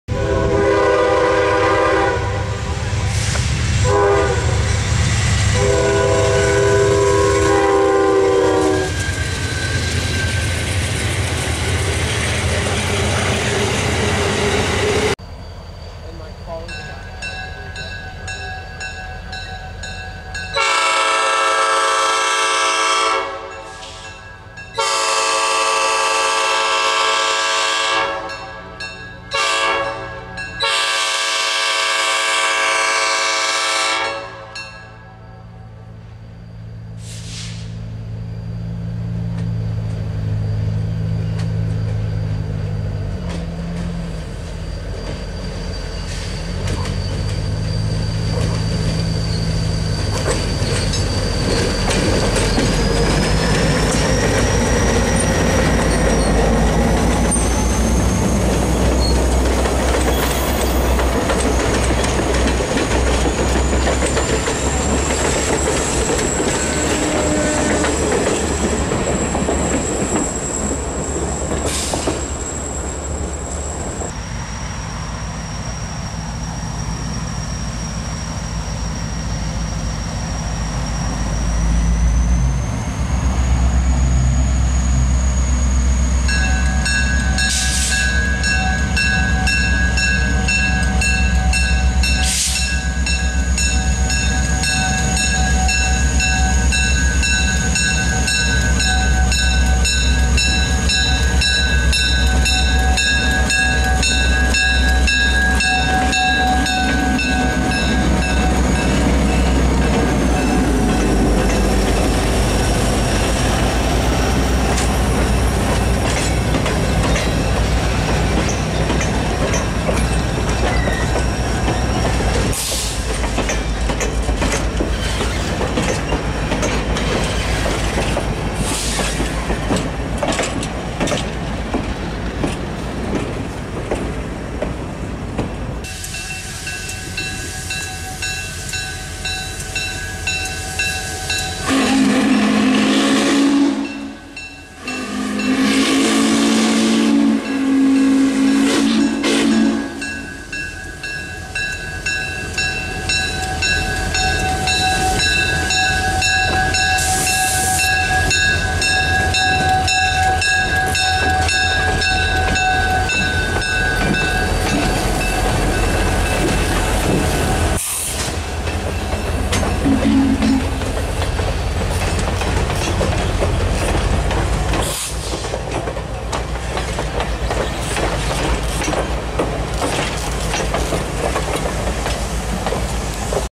North Carolina Transportation Museum: Southern 2601 leads the NCTM Passenger Train
Here at the North Carolina Transportation Museum in Spencer, NC, we have the Museum passenger train with lead unit Southern 2601 doing the honors. We first begin at the front of the Museum on the north side next to the signal bridge. Next, we set up on the other side of the pedestrian walkway to catch 2601 climbing the grade as he makes the sharp turn. Then finally we wrap up near the roundhouse where we see 2601 pass the beautiful J611 sitting on the pass track getting ready for the next rounds of the photo shoot. Always good to see the good ole southern paint scheme. As always, enjoy!
Lead unit
Southern 2601 EMD GP30

Recorded 9/23/18
1:33 pm - 3:35 pm